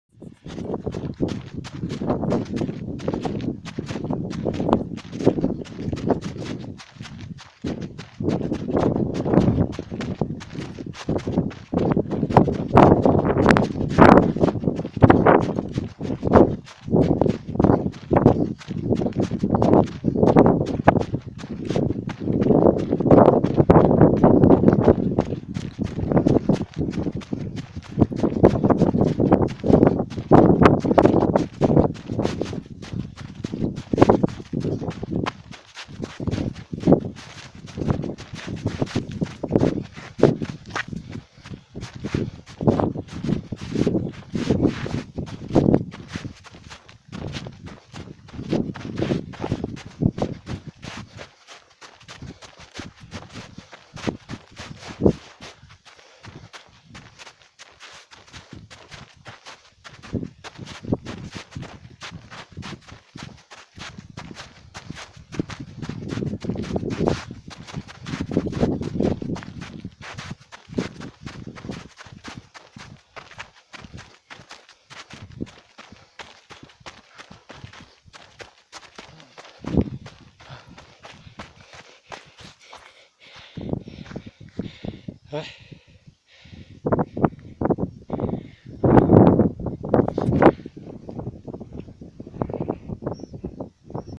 Bajada con nieve Puerto de Navaluenga
Bajada por pista tipo cortafuegos de acceso a las antenas del puerto de Navaluenga (Ávila) a unos 1900m de altitud.